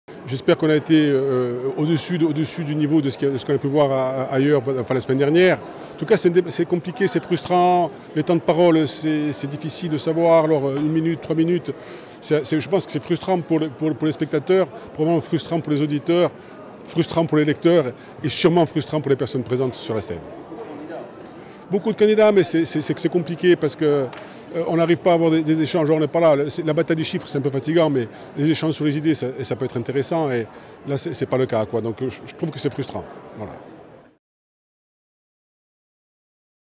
Municipales 2020 : Pierre Savelli revient sur le débat à Bastia
Pierre Savelli revient sur le débat à Bastia des Municipales 2020